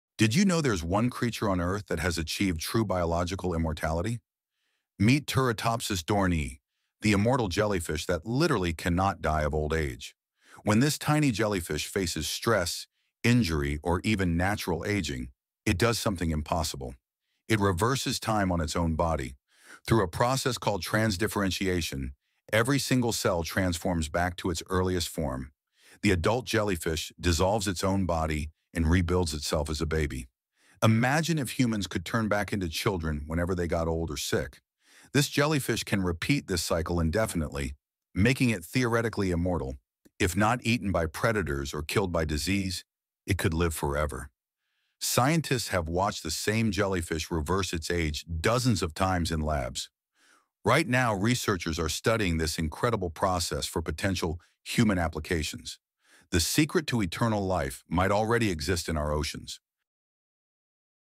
This Jellyfish Found the Secret to Immortality
immortal jellyfish can literally reverse aging forever